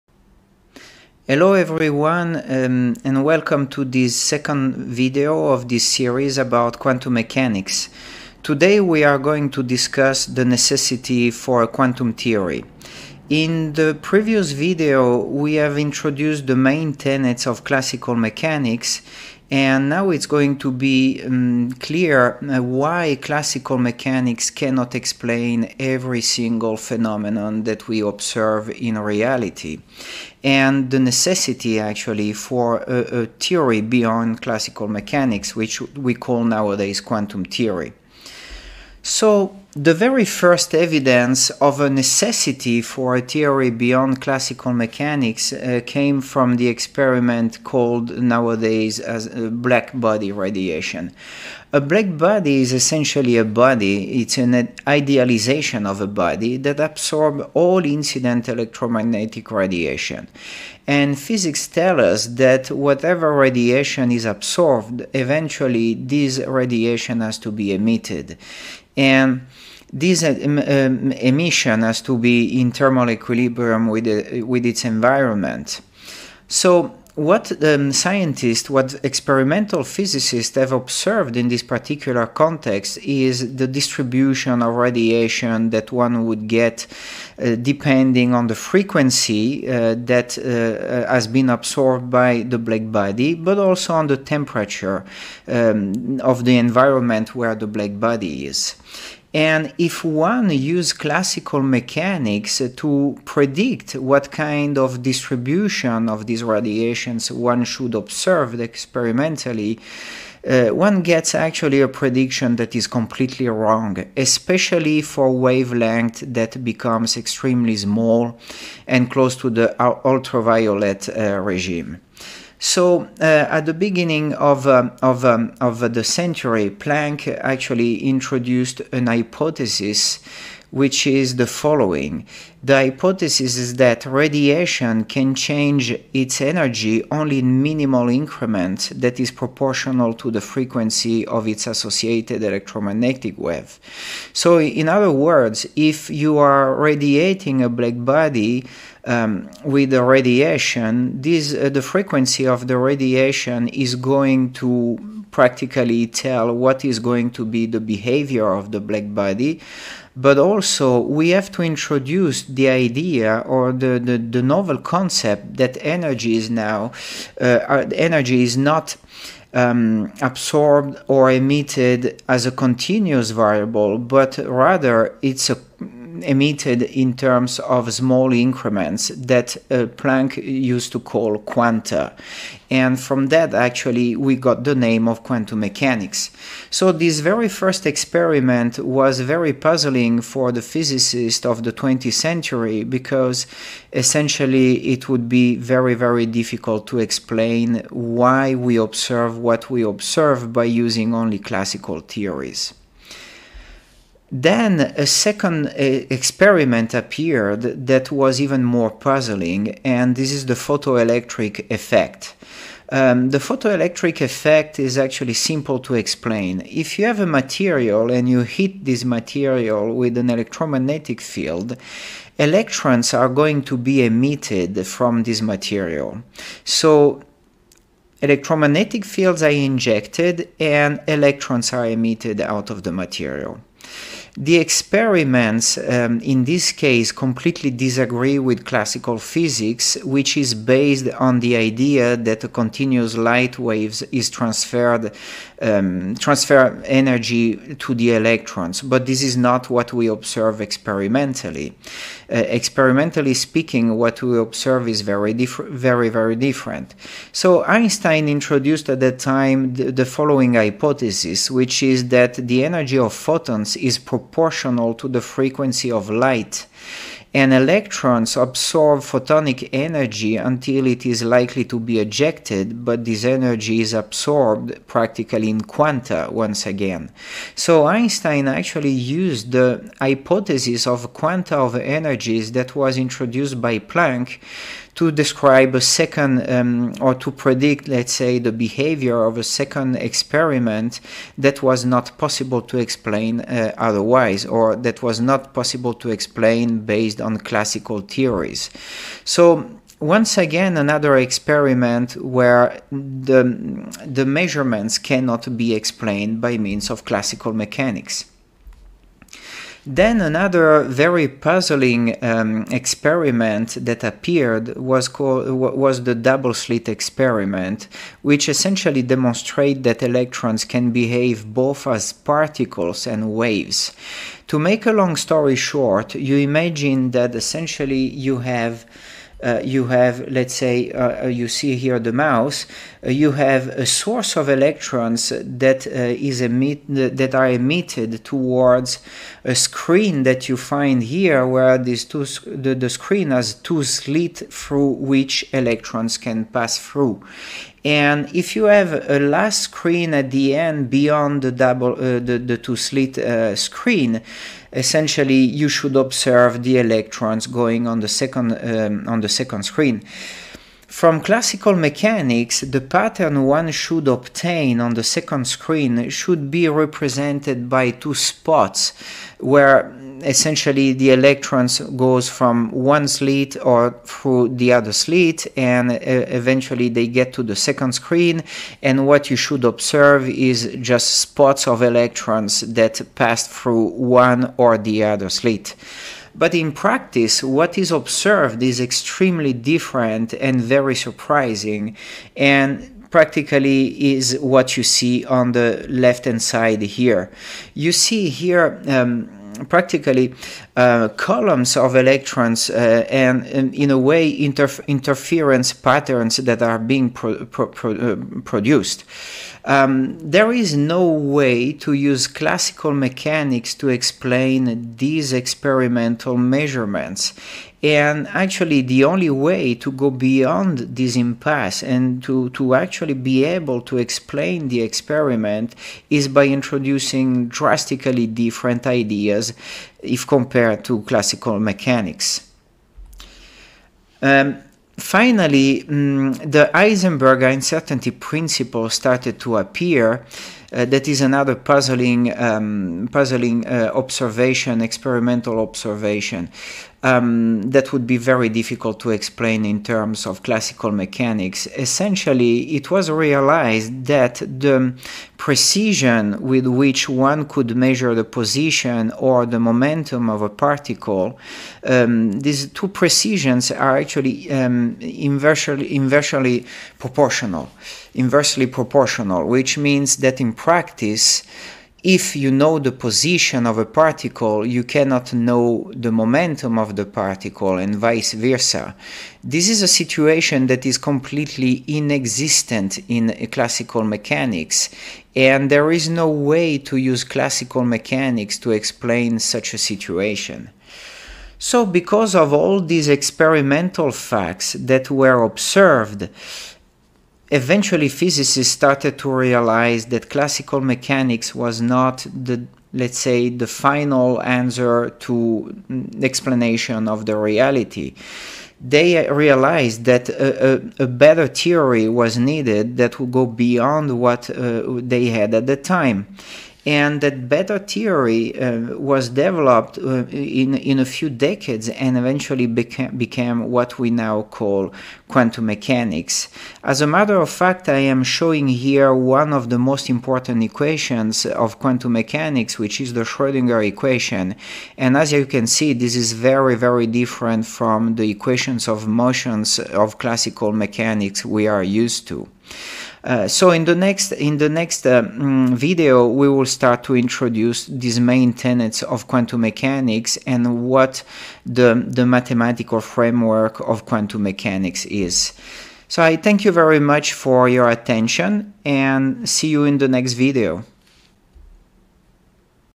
Quantum mechanics - Lesson 2 - The necessity for a quantum theory
In this video, part of a series of short lessons on quantum mechanics, the main experiments showing the inadequacy of classical mechanics are introduced and shortly described. The goal of this video is to clearly show the importance of a theory which goes beyond classical mechanics.